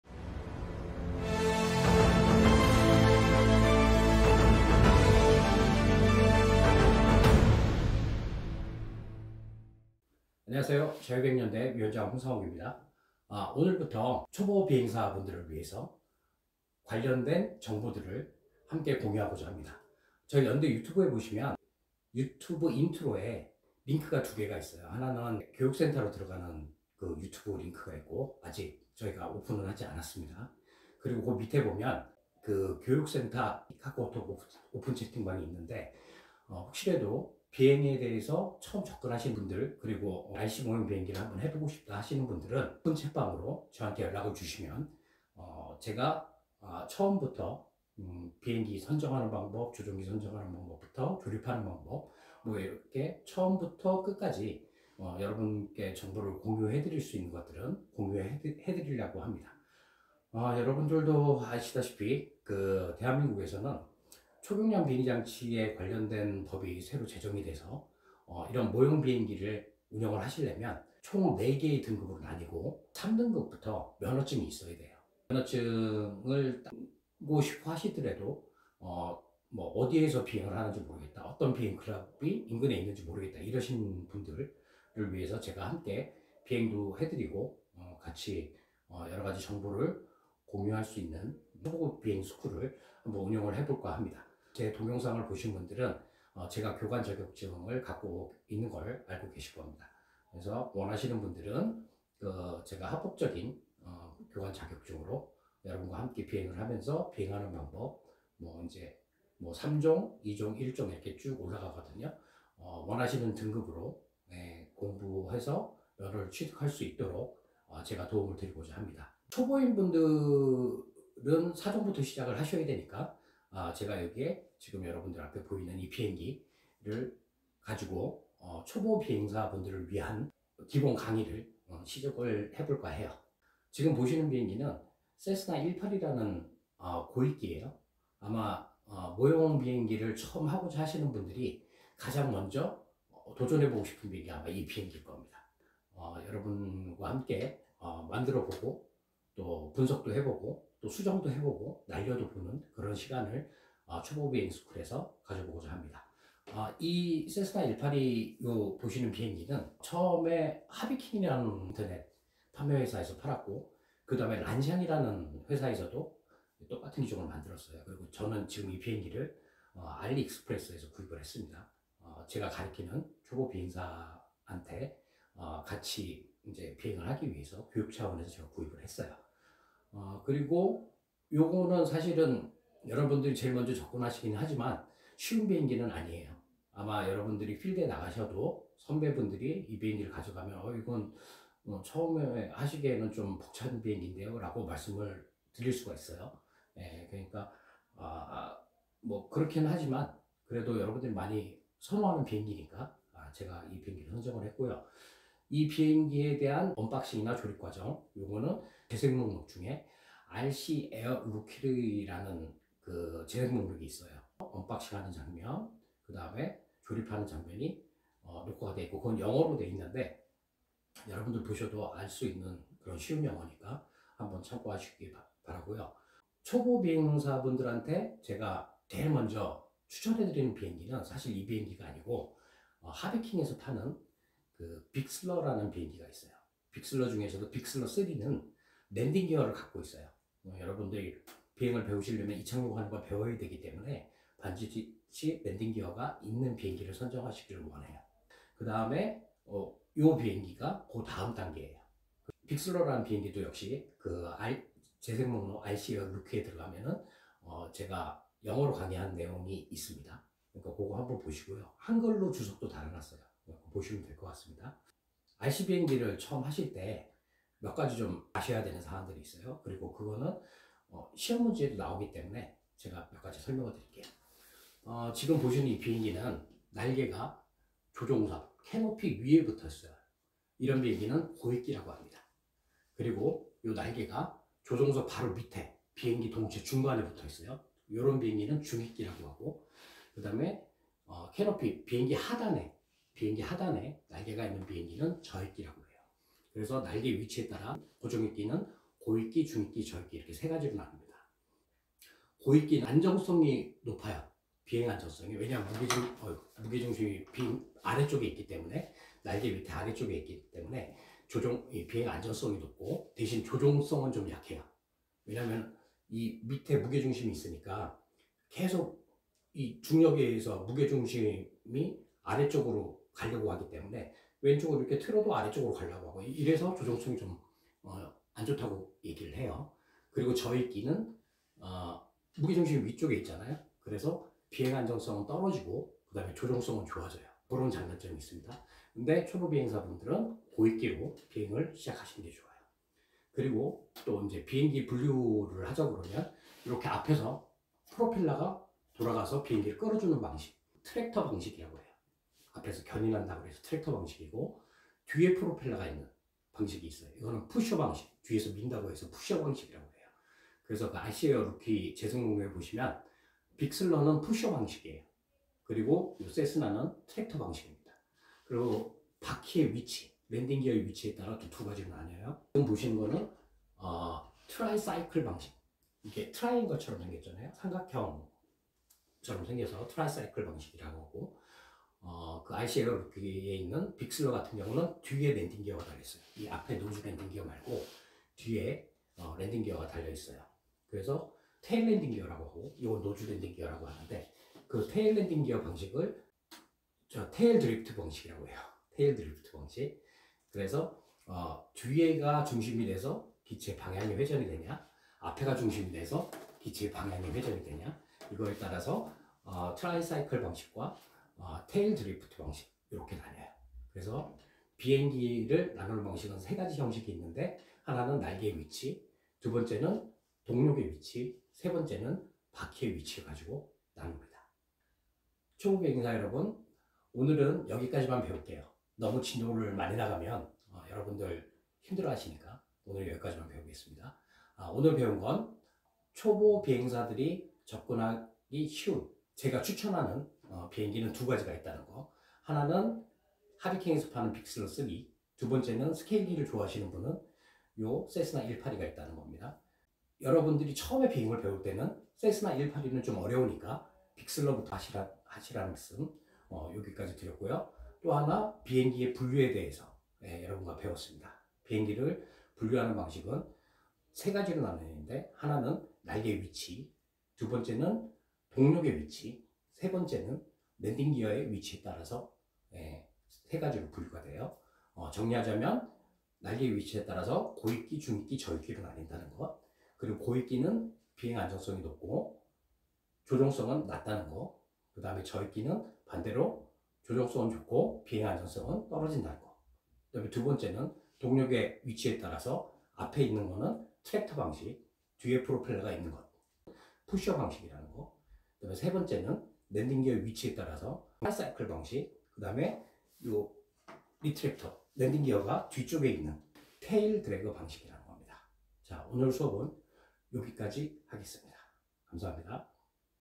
01. 초보비행스쿨 비행전 점검사항 - RC비행기알아보기[Eng Sub.]
모형RC비행기를 배우고자 하시는 분들을 위한 입문교육자료 입니다.
첫 번째 시간은, 모형RC비행기의 분류에 대해 알아보겠습니다. 샘플기체는 초보비행사 분들이 제일 관심을 갖는 세스나 182라는 기종입니다.

(초보비행사 실기 및 실습 교육문의)
- 카카오 오픈챗방 : FFCA Edu-center 채팅요청